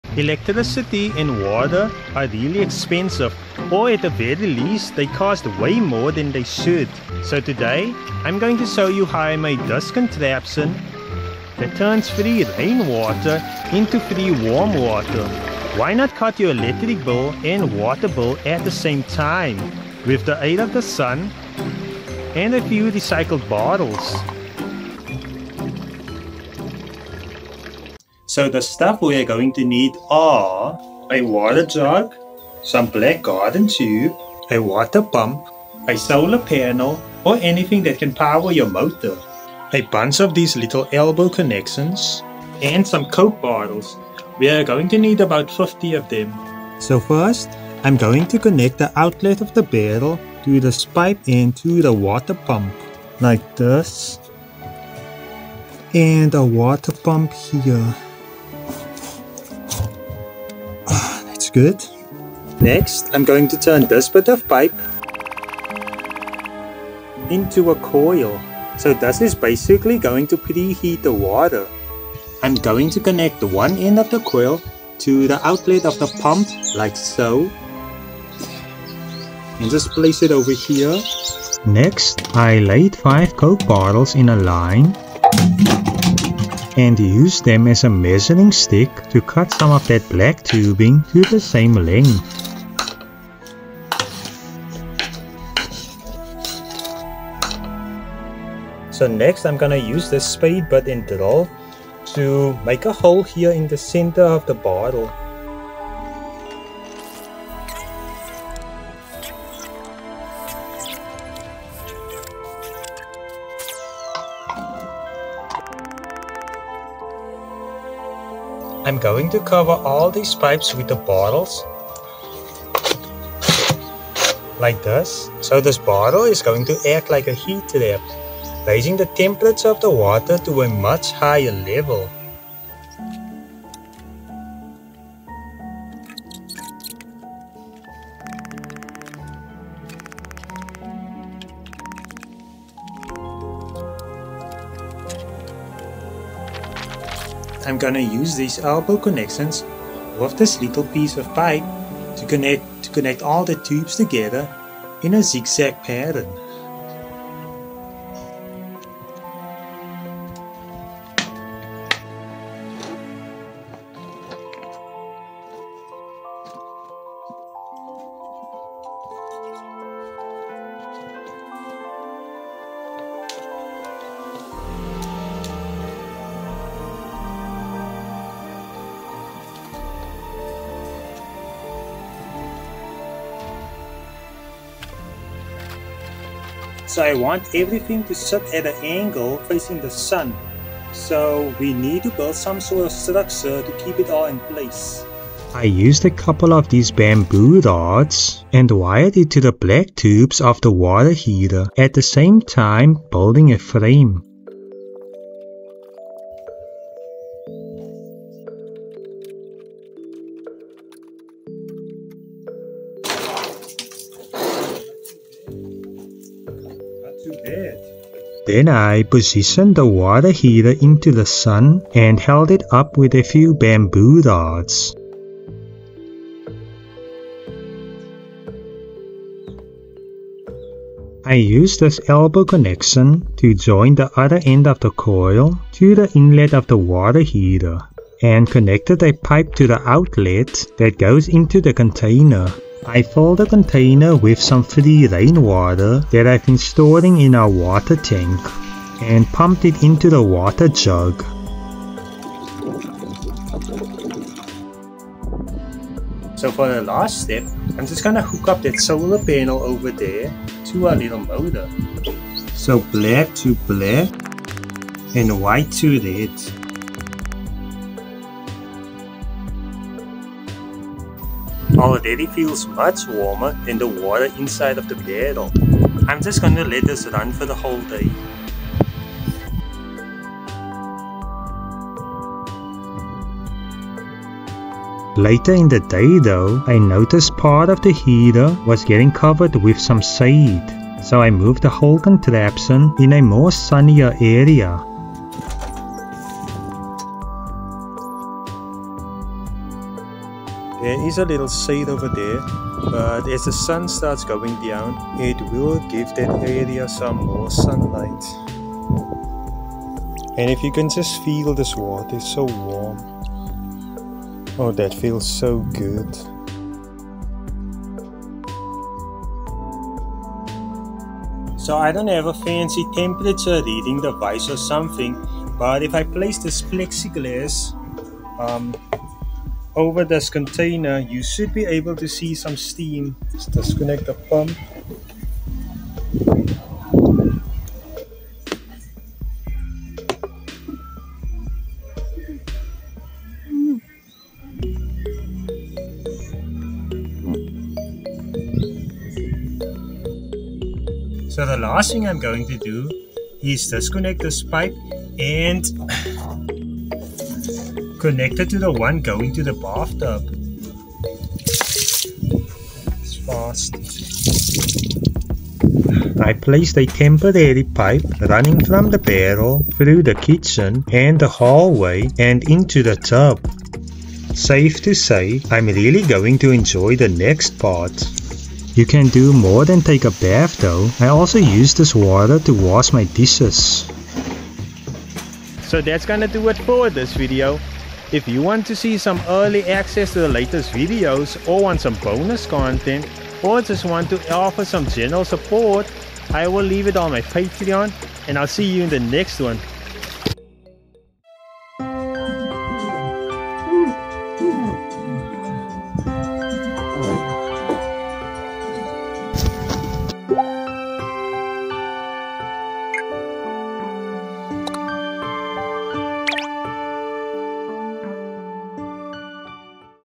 How to make a solar heater /Free Hot Water
Today I'm going to show you how I turned free rainwater into free warm water Buildng my own solar heating system, why not save money by cutting your electric and water bill at the same time.

Hope you enjoyed the film ;)